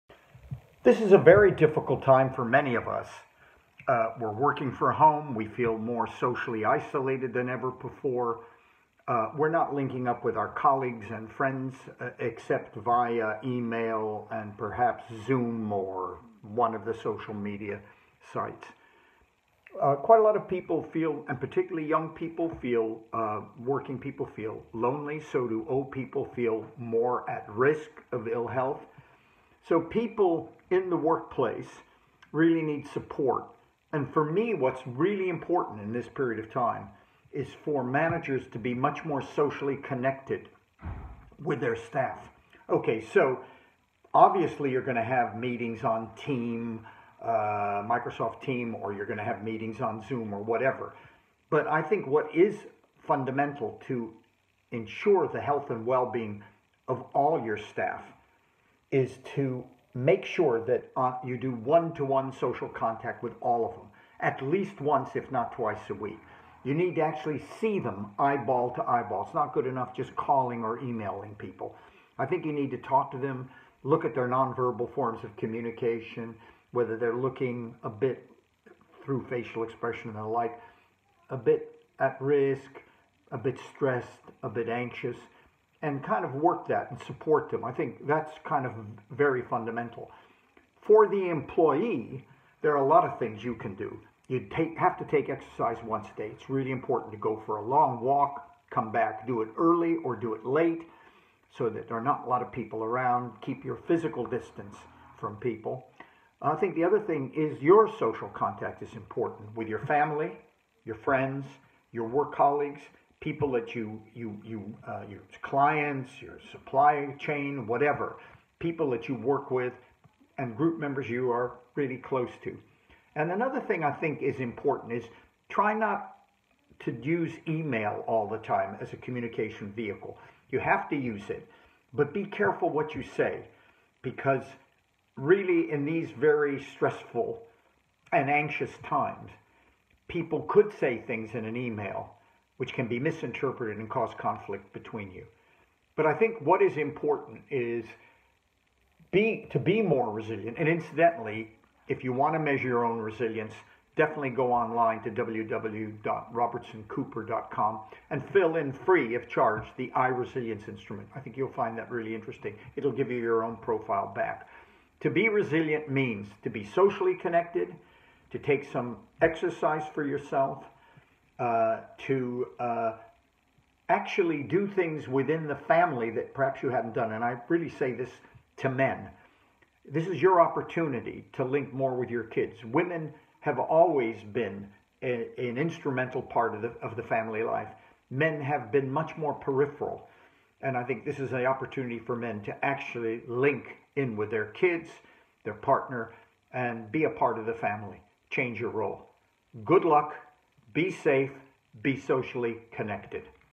Resilience during the COVID-19 Pandemic - Professor Sir Cary Cooper
Robertson Cooper’s Founding Director, Sir Cary Cooper, has been one of the media’s most in-demand commentators since the start of the pandemic.

Here, Cary discusses how both employers and employees can find resilience in an incredibly difficult time, with guidance on physical and mental health, and how we can remain socially connected despite the circumstances.